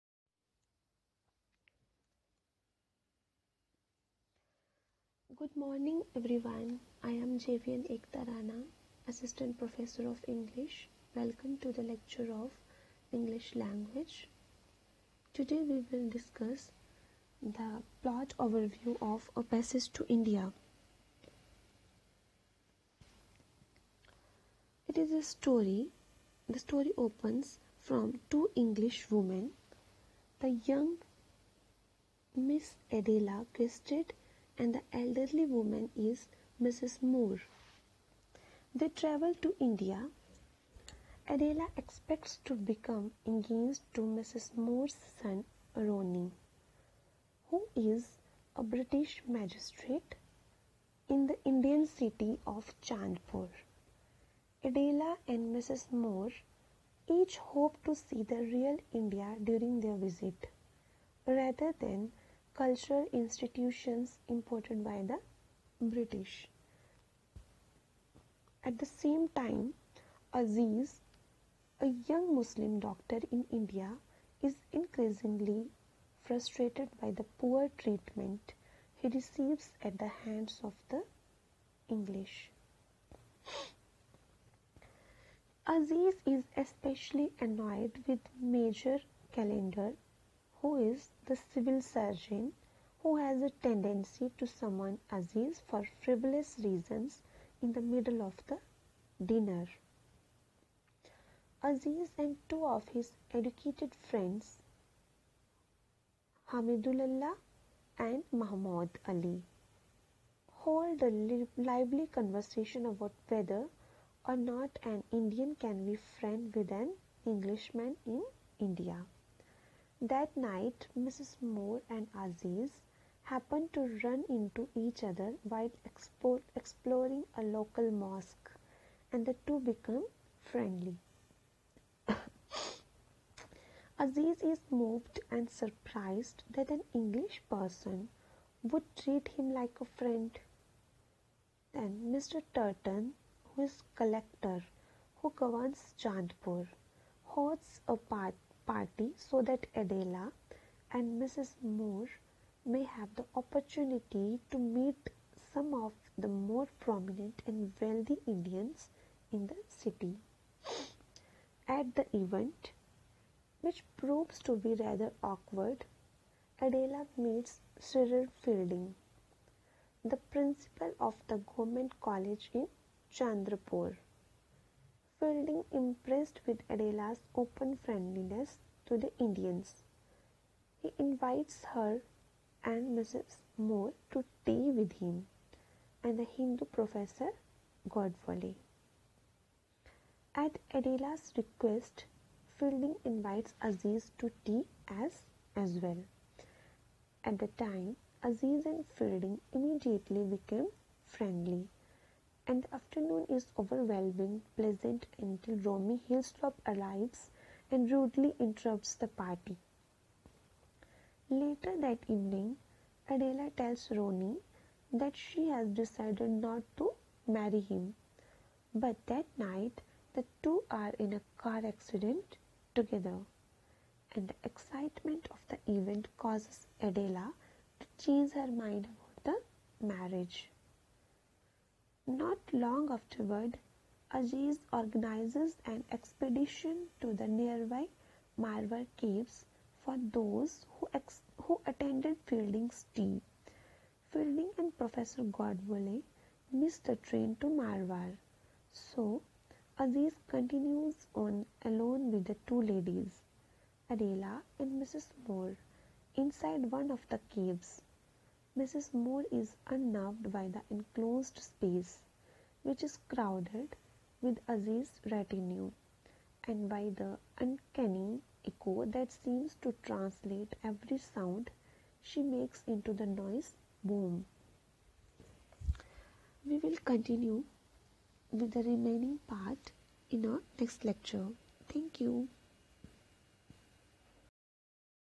JAYOTI VIDYAPEETH - Passage to India 1
JAYOTI VIDYAPEETH WOMEN UNIVERSITY JAIPUR
FACULTY OF Education and Methodology
FACULTY NAME: Ekta Rana
TOPIC NAME Passage to India 1